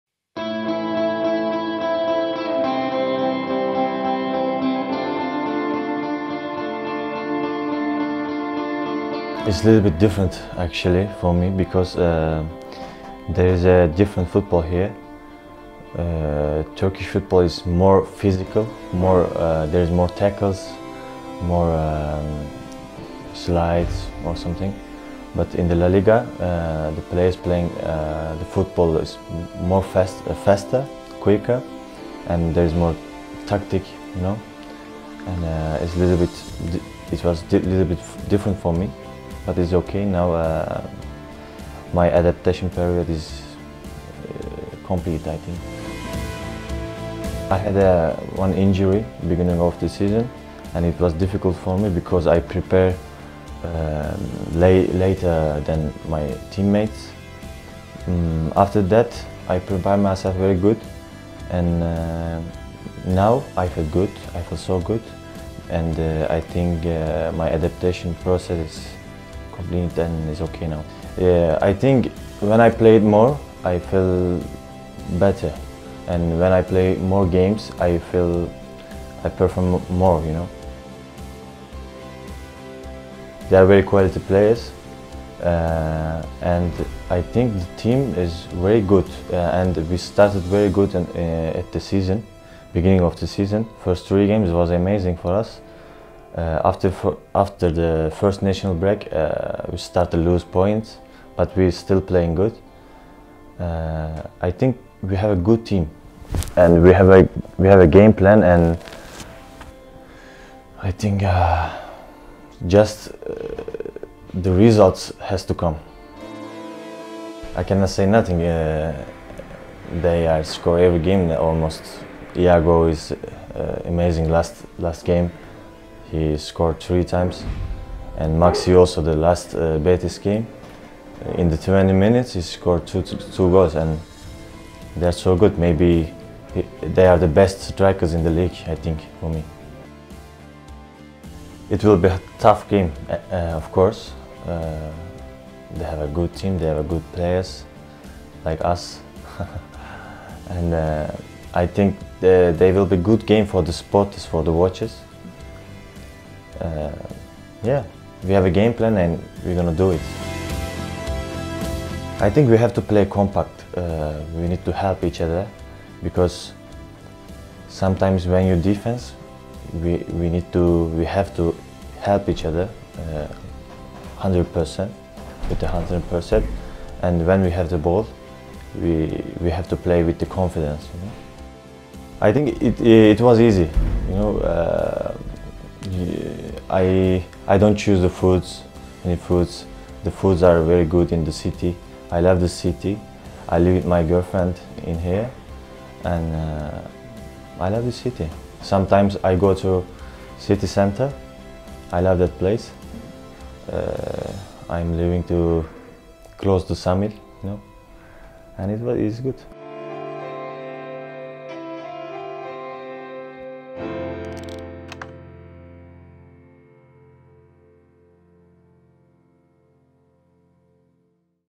Okay Yokuslu: "Good results will come"
Celta Media interviews international Turkish player Okay Yokuslu. Celta Media entrevista al internacional turco Okay Yokuslu: «Los buenos resultados van a llegar».